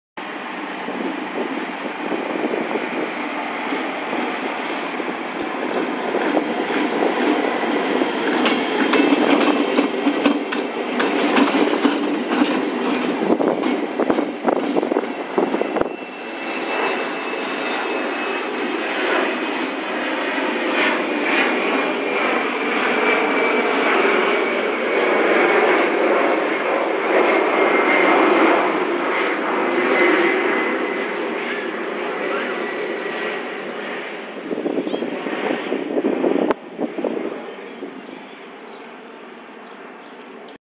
G22 7716 de Trenes Arg. Maniobrando en Saldias
En este video podran ver a la locomotora G22 7716 maniobrando en la playa Saldias del Belgrano Norte.